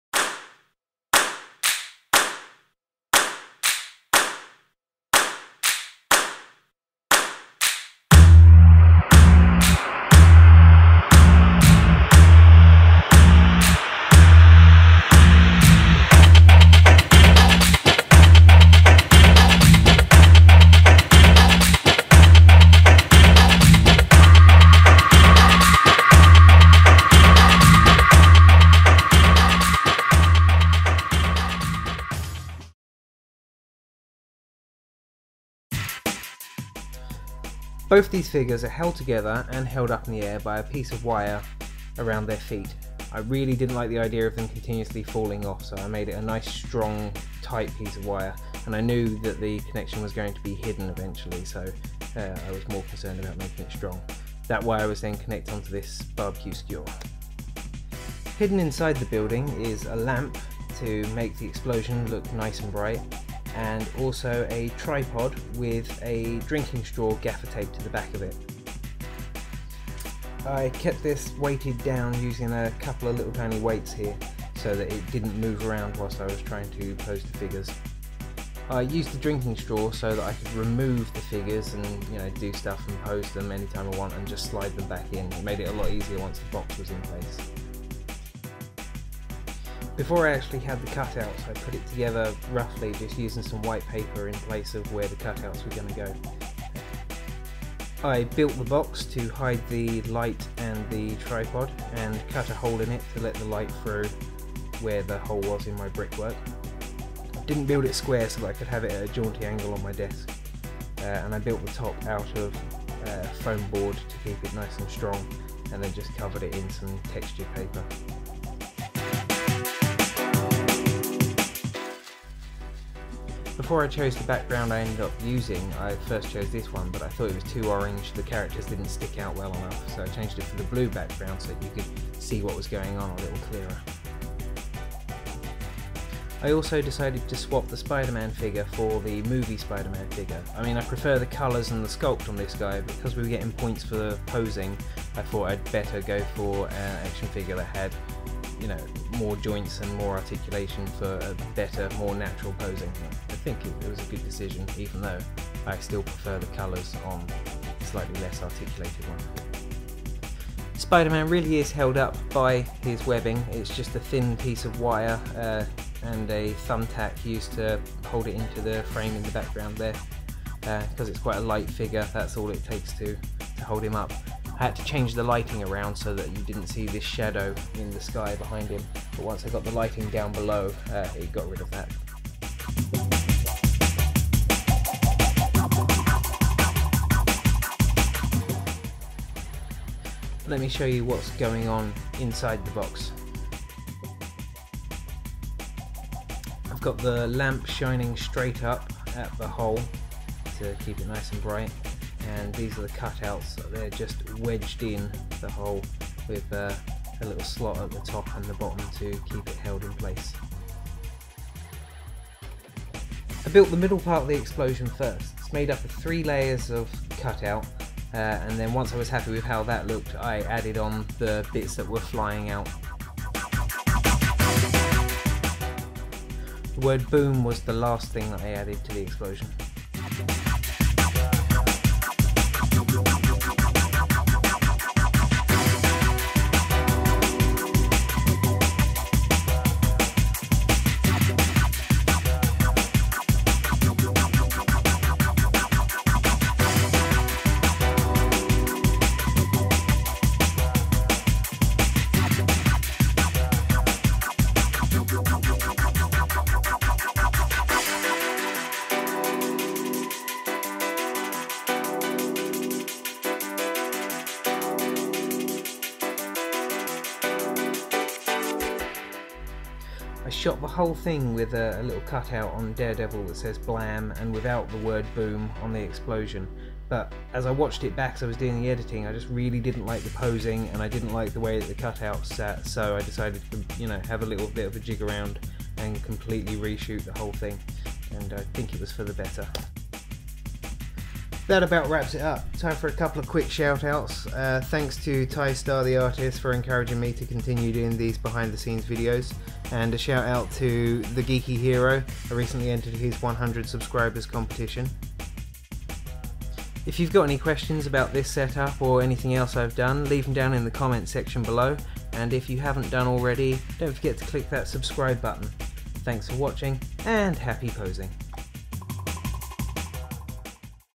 Round 3 BTS
A look at how I set up my entry into the 3rd round of the 2013 ACBA world championship tournament.